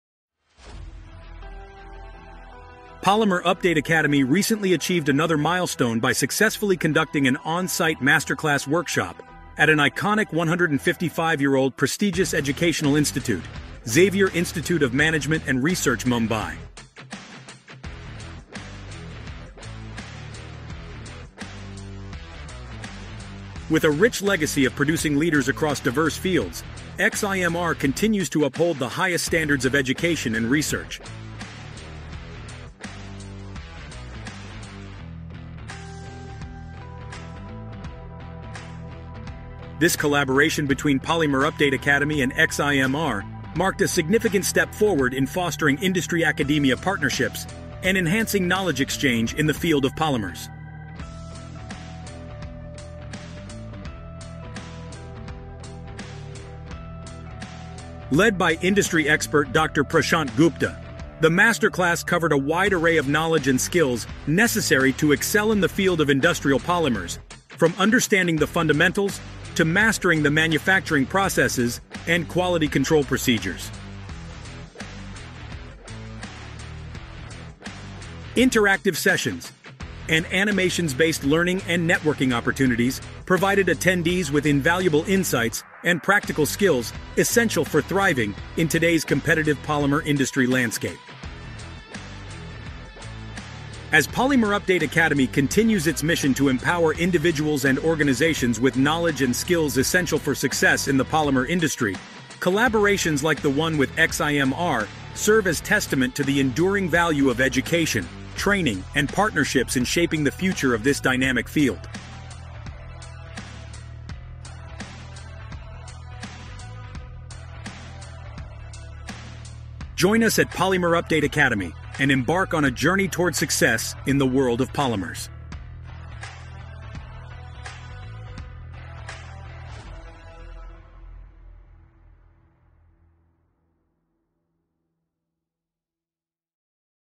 Mastering Polymers: Polymerupdate Academy Masterclass at XIMR, Mumbai
We are thrilled to share that Polymerupdate Academy recently achieved one more milestone. We've successfully concluded an On-site Masterclass on ' Industrial Polymer Technology - Materials, Manufacturing, and Quality Control' at The iconic Xavier Institute of Management & Research, Mumbai on February 29th, 2024.

Participants had the unique opportunity to learn from our lead Trainer, Dr. Prashant Gupta, and gain comprehensive knowledge and skills necessary to excel in the field of industrial polymers, from understanding the fundamentals to mastering the manufacturing processes and quality control procedures.

We extend our heartfelt congratulations to all participants who successfully completed the Masterclass. Your commitment, curiosity, and passion for automotive polymers truly impressed us. We are confident that the knowledge gained during this program will empower you to make a significant impact as you march ahead in your professional journey.